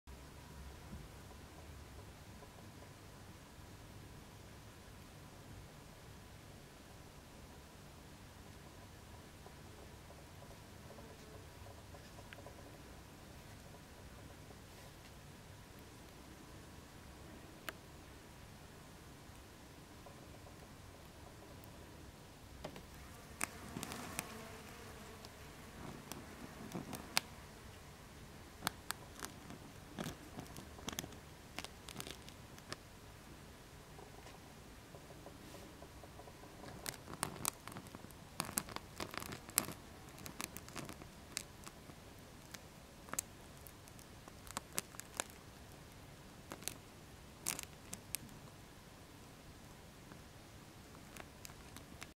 Fruit fly larvae and adults on a decaying banana
Fruit fly (quite possibly Drosophila melanogaster) larvae and new adults on a decaying banana. Reproduction has been successful!

Mahlakärpäsen (hyvin mahdollisesti Drosophila melanogaster) toukkia ja uusia, vähintäänkin toisen sukupolven aikuisia ylikypsällä banaanilla.